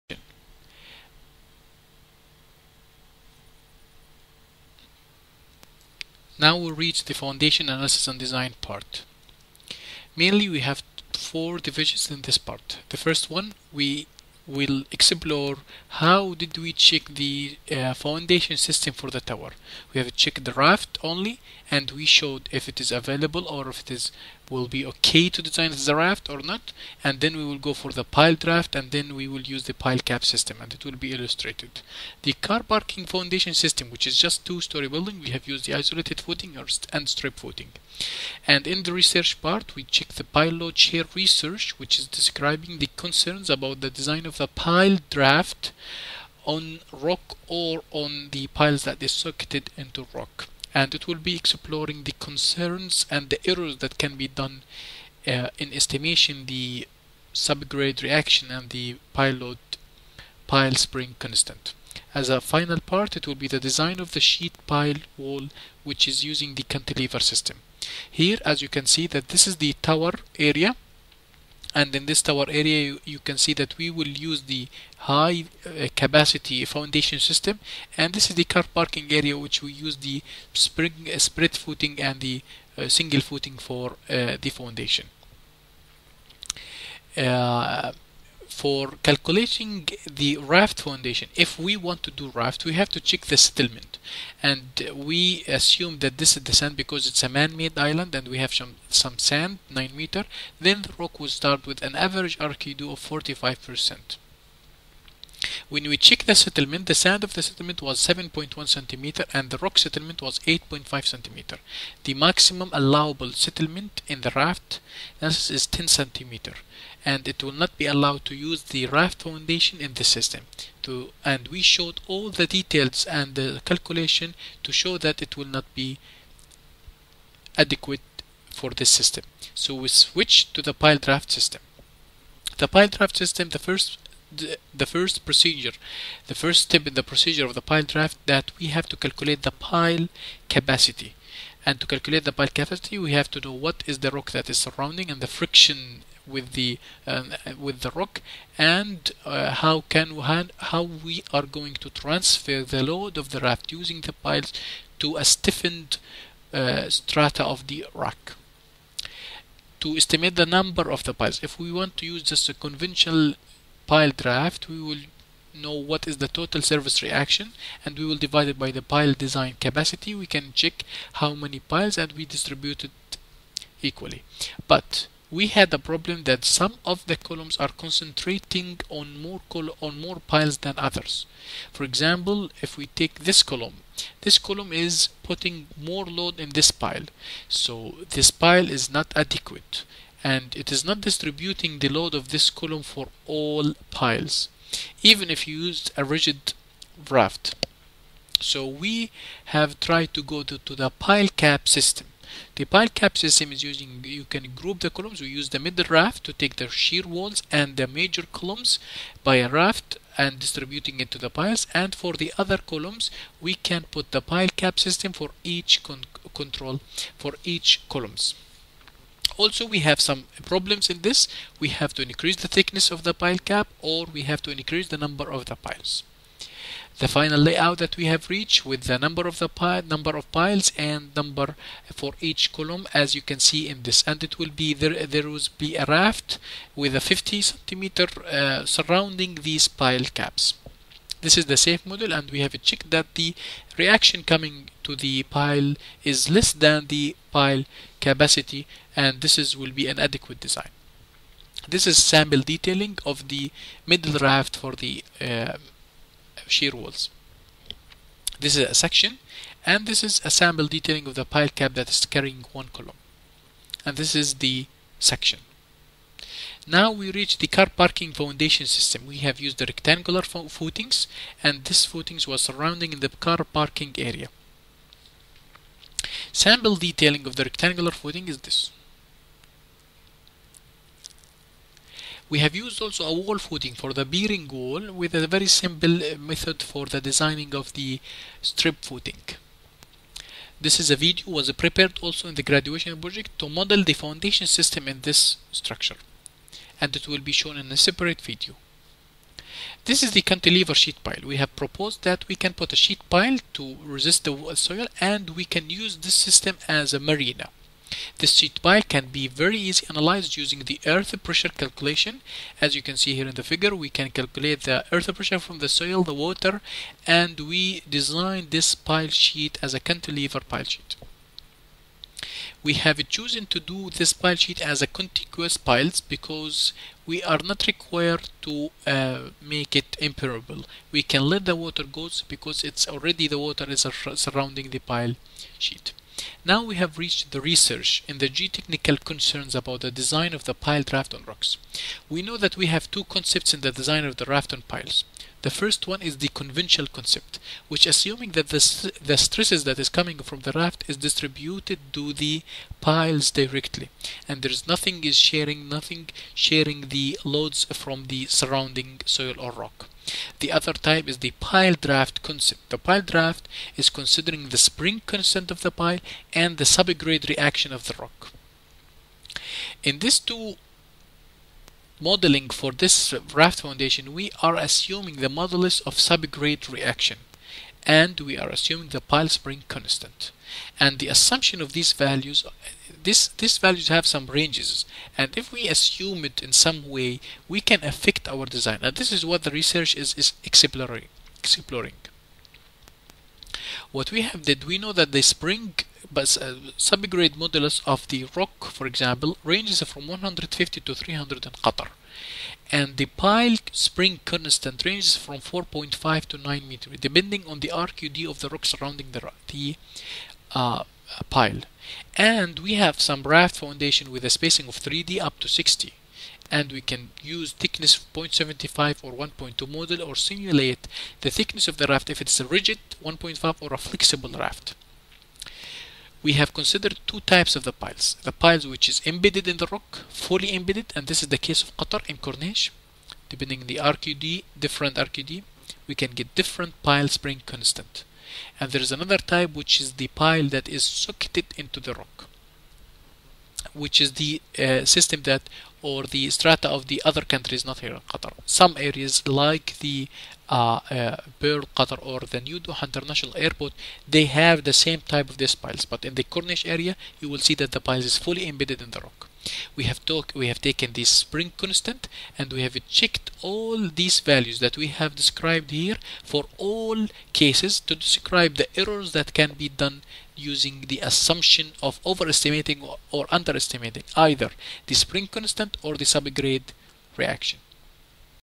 Graduation Project: Structure, Foundation, Project Management - Part 3
This Graduation Project is presented by Eng. Haytham Sadeq, Eng. Mohammed Saleem Taha & Eng. Rakan Amr
and it was divided into main three divisions; the structural analysis and design, the foundation analysis and design and the project management and scheduling.
This Graduation Project was presented in Civil Engineering Department in Qatar University, and it was supervised by Dr. Khaldoon Bani-hani, Dr. Ahmed Al-senouci and Dr. Hisham T. Eid.